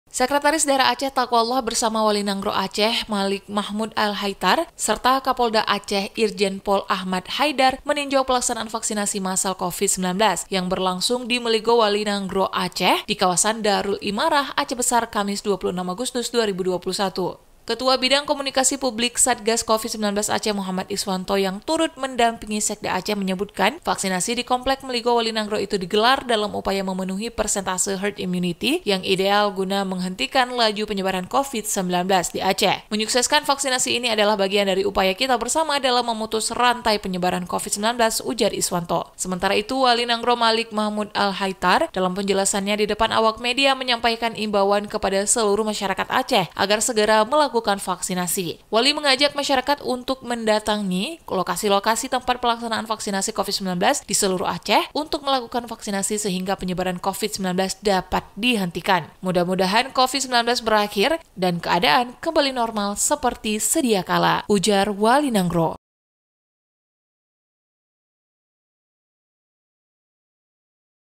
Sekda Aceh Dampingi Wali Nanggroe dan Kapolda Tinjau Vaksinasi Covid 19
ACEH BESAR – Sekretaris Daerah (Sekda) Aceh Taqwallah mendampingi Wali Nanggroe Aceh Malik Mahmud Al-Haythar serta Kapolda Aceh Irjen Pol Ahmad Haydar meninjau pelaksanaan vaksinasi massal Covid-19 yang berlangsung di Meuligoe Wali Nanggroe Aceh, di kawasan Darul Imarah Aceh Besar, Kamis 26 Agustus 2021.

Sekda Taqwallah tiba di depan kediaman Wali Nanggroe pukul sembilan pagi dan langsung disambut oleh Malik Mahmud serta Irjen Pol Ahmad Haydar. Ketiganya kemudian berjalan menuju lokasi vaksinasi yang juga terletak di komplek tersebut.

Ketua Bidang Komunikasi Publik Satgas Covid-19 Aceh Muhammad Iswanto yang turut mendampingi Sekda Aceh menyebutkan, vaksinasi di komplek Meuligoe Wali Nanggroe itu digelar dalam upaya memenuhi persentase herd immunity yang ideal guna menghentikan laju penyebaran Covid-19 di Aceh.

“Menyukseskan vaksinasi ini adalah bagian dari upaya kita bersama dalam memutus rantai penyebaran Covid-19,” ujar Iswanto.

Sementara itu, Wali Nanggroe Malik Mahmud dalam penjelasannya di depan awak media menyampaikan imbauan kepada seluruh masyarakat Aceh agar segera melakukan vaksinasi.

Wali mengajak masyarakat untuk mendatangi lokasi-lokasi tempat pelaksanaan vaksinasi Covid-19 di seluruh Aceh untuk melakukan vaksinasi sehingga penyebaran Covid-19 dapat dihentikan.

“Mudah-mudahan Covid-19 berakhir dan keadaan kembali normal seperti sediakala,” ujar Wali.